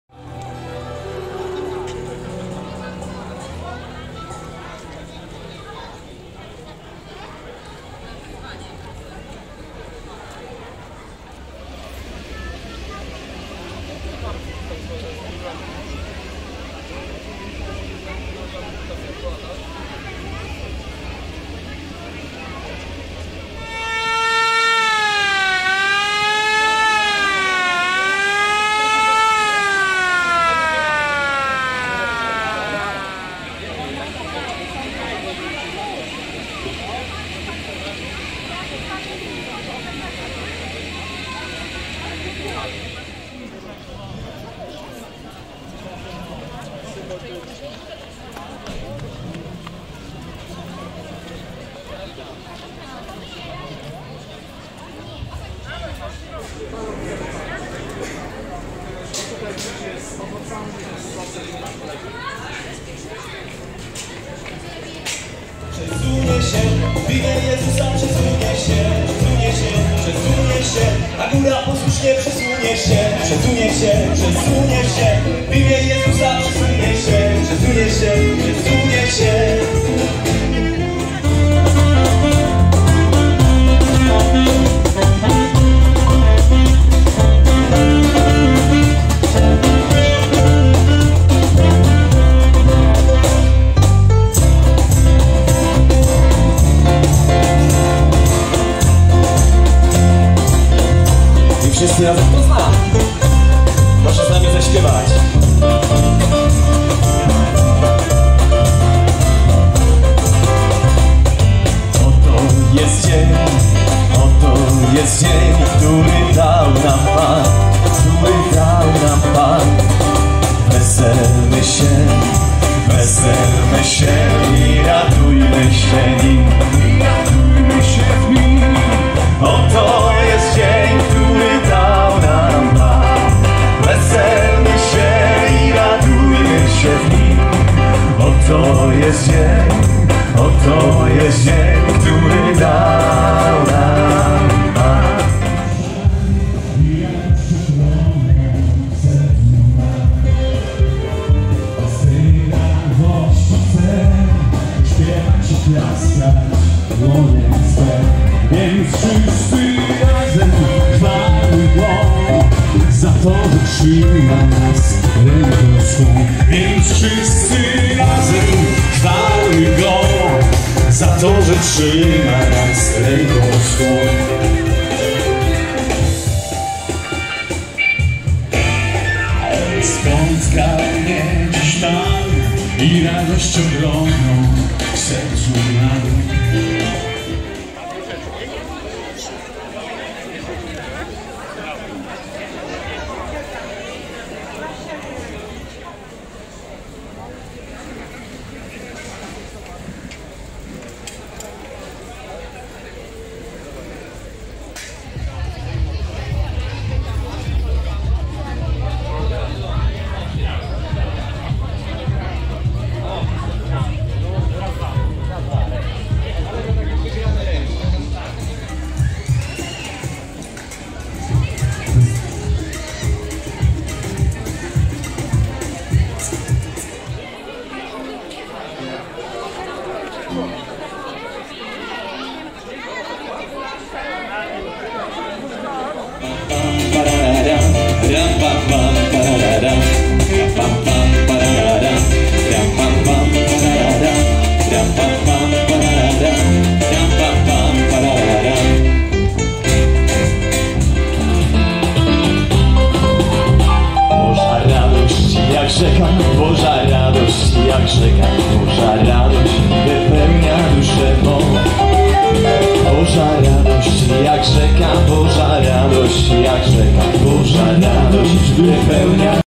Festyn Parafialny 2022
Festyn Parafialny z okazji 109. rocznicy erygowania Parafii św. Kazimierza w Pruszkowie